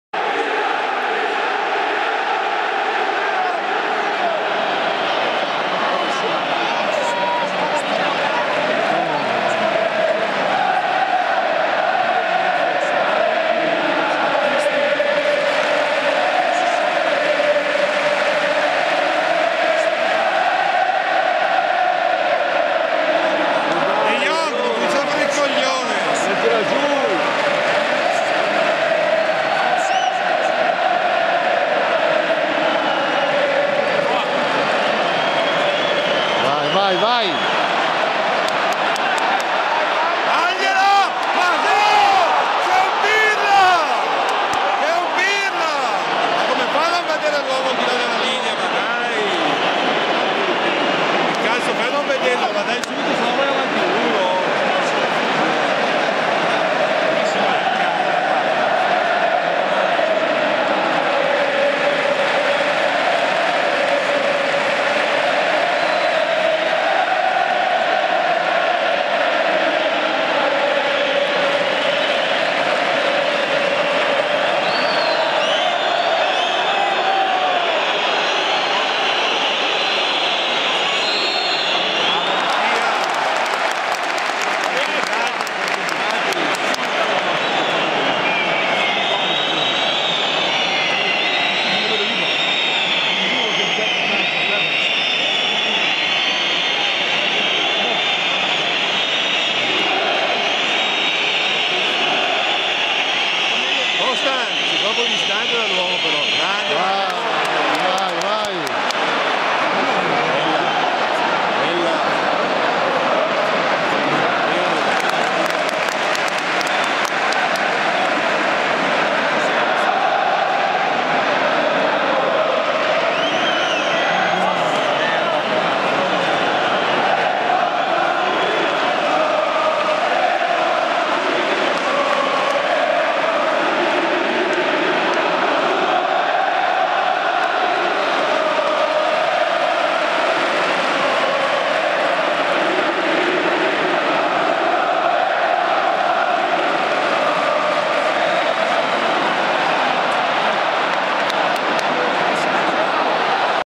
Milan Juventus 1-0 ''GRAN TIFO IN CURVA'' IN FULL HD'' .
Milano 25 Novembre 2012 Milan Juventus 1-0 momenti in Curva Sud Milano prima e durante la partita.